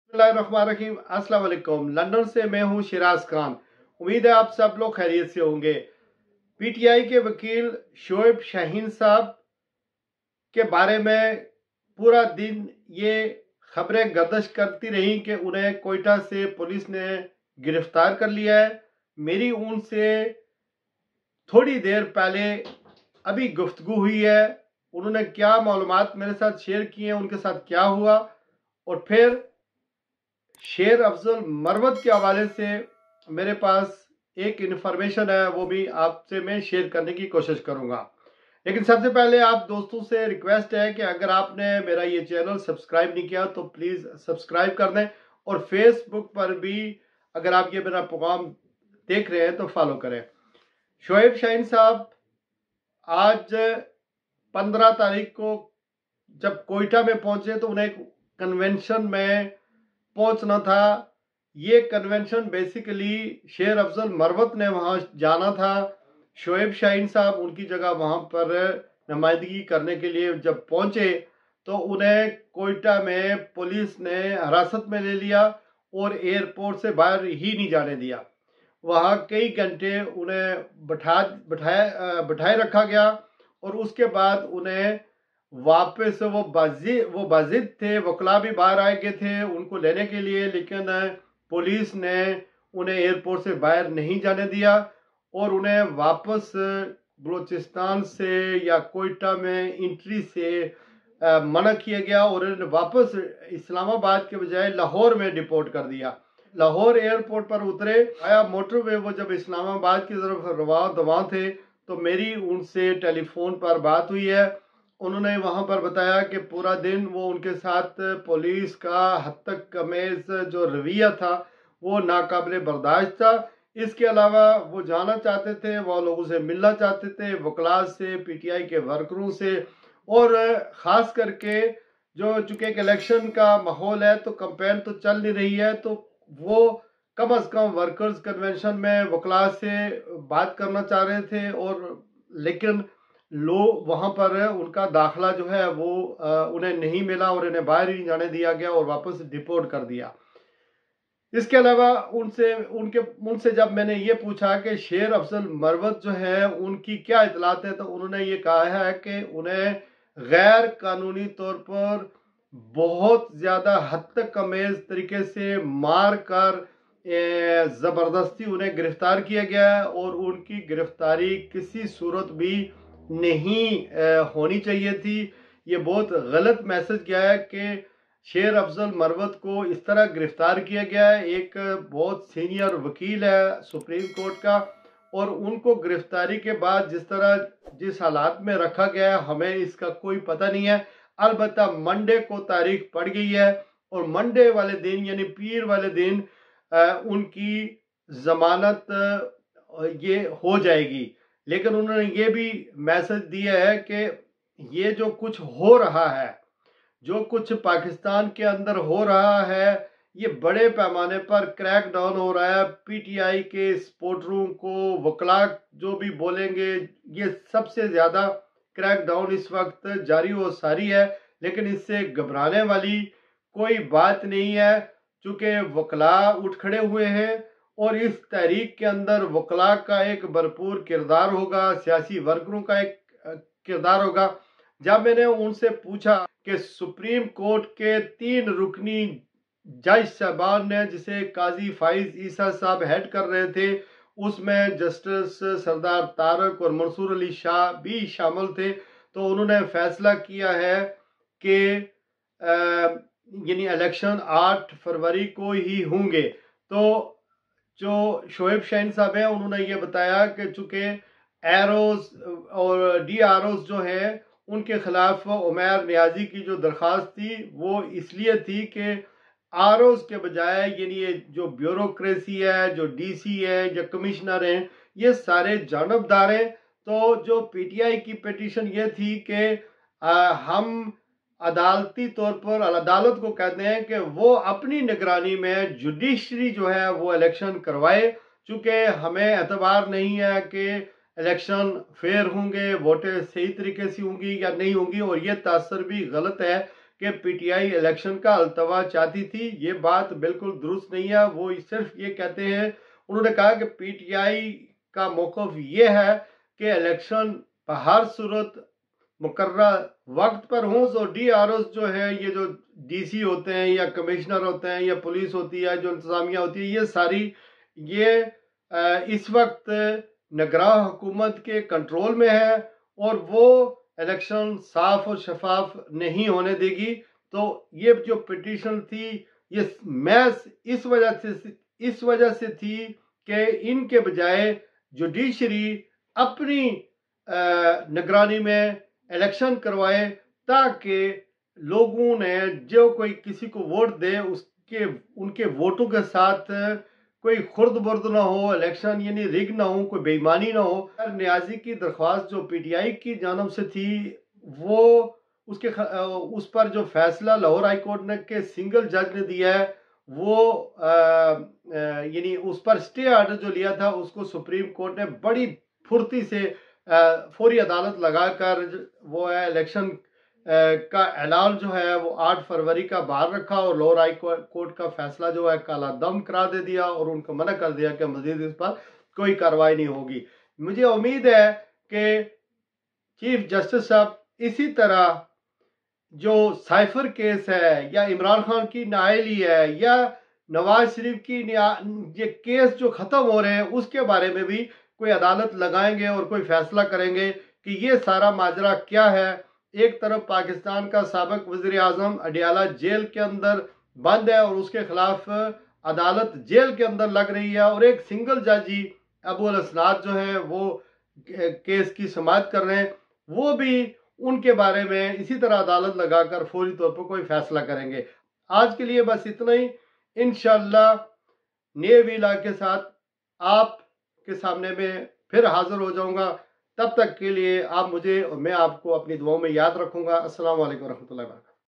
Shoeb Shaheen PTI What happend with him in Quetta? شیر افضل مروت کی ضمانت کب ہوگئی ؟
شعیب شاہین پی ٹی آئی کے وکیل کوئیٹہ میں کنونشن میں شرکت کرنے گئے وہاں ان کے ساتھ کیا ہوا؟
شیر افضل مروت ایڈووکیٹ کی ضمانت ہوگی؟
چیف جسٹس کی پھرتیاں